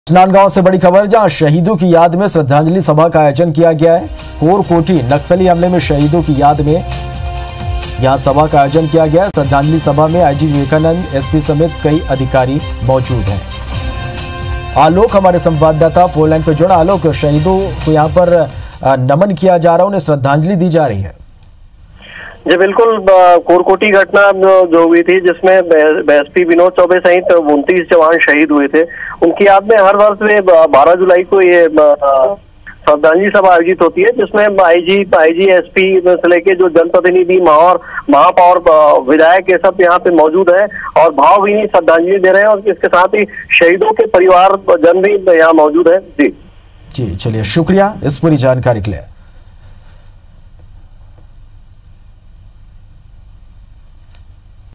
शहीदों की याद में श्रद्धांजलि सभा | SP Vinod Chaubey समेत 29 जवान नक्सली हमले में हुए थे शहीद
IBC24, Rajnandgaon, शहीदों की याद में श्रद्धांजलि सभा, SP Vinod Chaubey समेत 29 जवान नक्सली हमले में हुए थे शहीद